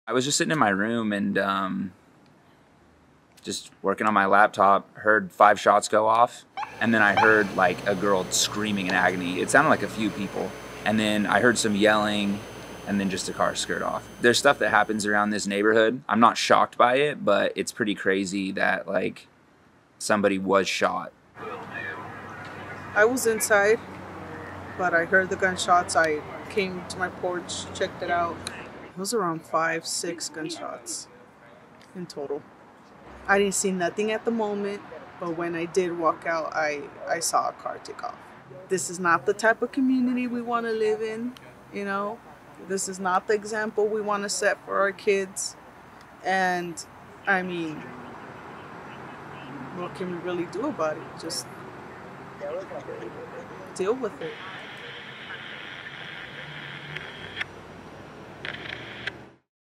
San Jose: 2 killed, 4 wounded in shooting near SJSU
Two people were killed in a shooting near San Jose State University late Tuesday, police said.

The shooting was reported on the 600 block of South 8th Street, near East Reed Street and two blocks south of the university, about 10 p.m., said San Jose police Sgt. Christian Camarillo.

LET’S CONNECT
Mercury News